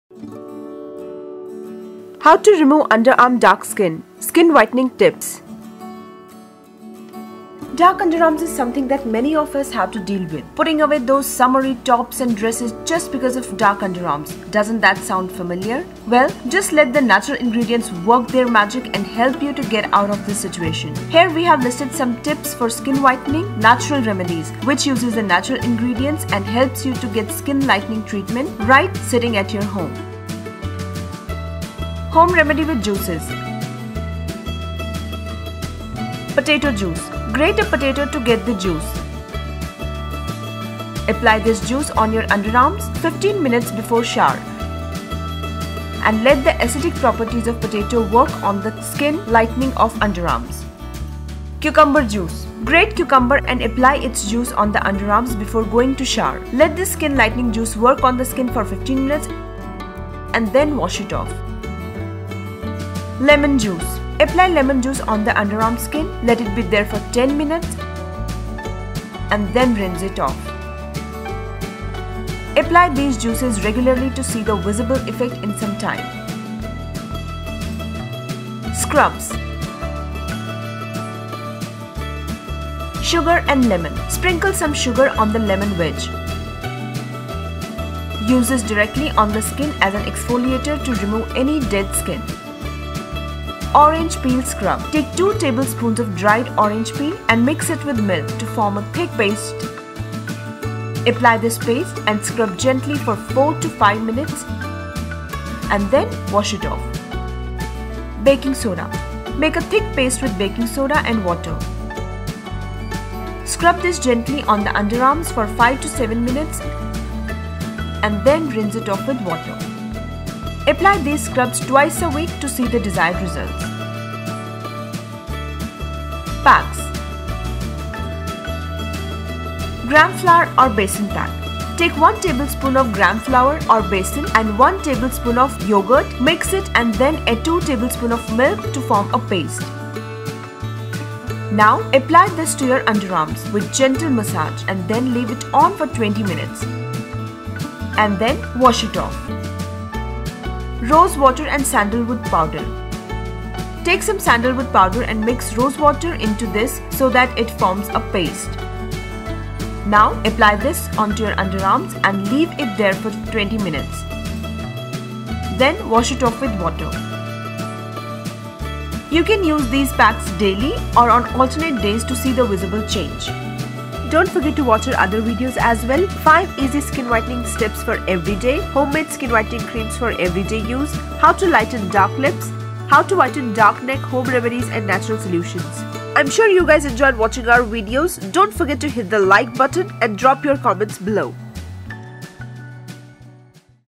How to Remove Under Arm Dark Skin - Skin Whitening Tips
There can a lot of factors leading to dark underarms like hair removal creams, shaving, extreme sweating, dead skin, etc. Here is a list of some simple Skin Whitening home remedies which are not just effective and safe but very affordable too. So keep on watching and say good bye to your dark underarms.

Dark underarms is something that many of us have to deal with. Putting away those summery tops or dresses just because of dark underarms, doesn’t that sound familiar ??? well  just let the natural ingredients work their magic and help you to get out of this situation. here we have listed some tips for skin whitening natural remedies  which uses the natural ingredients and helps you to get skin lightening treatment right sitting at your home

Home remedy with juices:-

Potato juice: grate a potato to get the juice .apply this juice on your underarms 15 minutes before shower and let the  acidic properties of potato work on the s kin lightening of underarms

Cucumber juice: grate cucumber and apply its juice on the underarms  before going to shower. Let the skin lightening juice work on the skin for 15 minutes  and then wash it off

Lemon juice: Apply lemon juice on the underarm skin. Let it be there for ten minutes and then rinse off
Apply these juices regularly to see the visible effects in some time

Scrubs:-

Sugar and lemon: Sprinkle some sugar on the lemon wedge. Use this directly on the  skin as an exfoliator to remove any dead skin.

Orange peel scrub: take 2 table spoons of dried orange peel and mix it with milk to form a thick paste. Apply this paste and scrub gently for 4 to 5 minutes and then wash it off.

Baking Soda: make a thick paste with baking soda and water. Scrub this gently on the underarms  for 5 to 7 minutes and then rinse it off with water.
Apply these scrubs twice a week to see the desired results.

Packs:-

Gram flour or besan pack:
Take 1 tablespoon of gram flour or besan  and one table spoon of curd mix it and then add 2 tablespoon of milk to form a paste. Now apply this to your underarms with gentle massage then leave it on for  20 minutes  and then wash it off.

Rose Water and Sandalwood Powder:
Take some sandalwood powder and mix rose water into this so that it forms a  paste. Now apply this on your underarms and leave it for 20 minutes. Then wash it off with water.

You can use these packs daily or on alternate days to see the visible change.